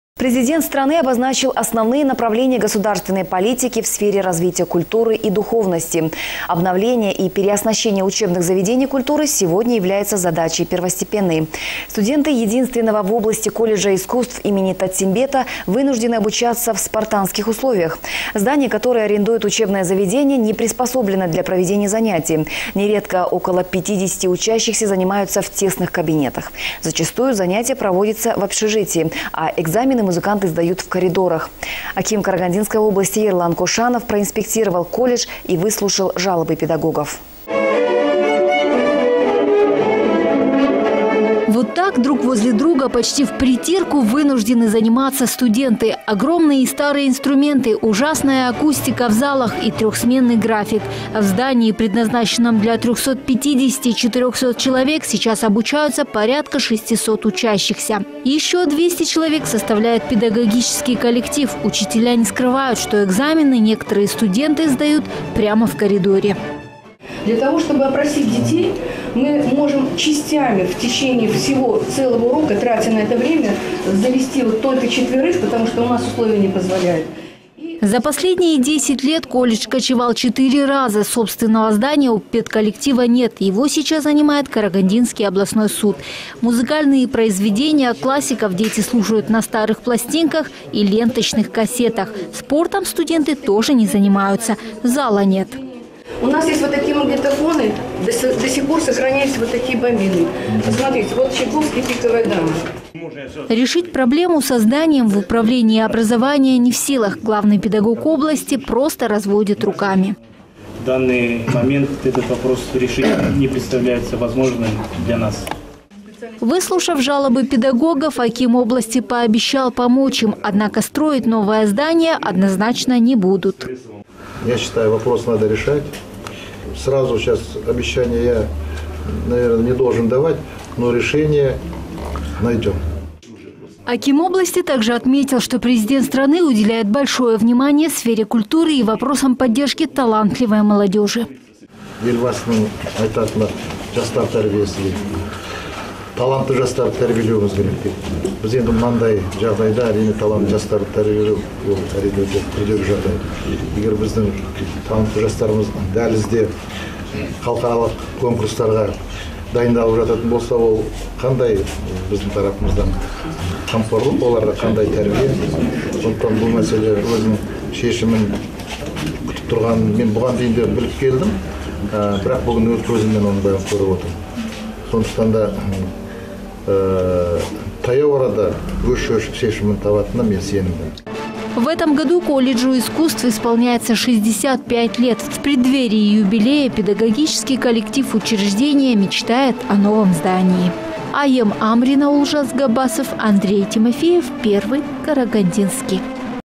Студенты карагандинского колледжа искусств имени Т.Казангапулы вынуждены учиться в три смены
Студенты единственного в области колледжа искусств имени Таттимбета вынуждены обучаться в спартанских условиях. Здание, которое арендует учебное заведение, раньше было центром переливания крови. Оно не приспособлено для проведения занятий. Нередко около 50 учащихся занимаются в тесных кабинетах, которые вмещают только 15-20 человек. Зачастую занятия проводятся в общежитии. А экзамены музыканты сдают в коридорах. Сегодня аким области проинспектировал колледж и выслушал жалобы педколлектива.
Вот так, друг возле друга, почти впритирку вынуждены заниматься студенты. Огромные и старые инструменты, ужасная аккустика в залах и трехсменный график. В здании, предназначенном для 350-400 человек, сейчас обучаются порядка шестисот учащихся. Еще 200 человек составляет педагогический коллектив. Учителя не скрывают, что экзамены некоторые студенты сдают в коридоре.
За последние 10 лет колледж кочевал четыре раза. Собственного здания у педколлектива нет, его сейчас занимает карагандинский областной суд. Музыкальные произведения классиков дети слушают на старых пластинках и ленточных кассетах. Спортом студенты тоже не занимаются. Зала нет.
Решить проблему со зданием в управлении образования не в силах. Главный педагог области просто разводит руками.
Выслушав жалобы педагогов, аким области пообещал помочь им. Однако строить новое здание однозначно не будут.
Аким области также отметил, что Президент страны уделяет большое внимание сфере культуры и вопросам поддержки талантливой молодежи.
В этом году колледжу искусств исполняется 65 лет. В преддверии юбилея, педагогический коллектив учреждения мечтает о новом здании.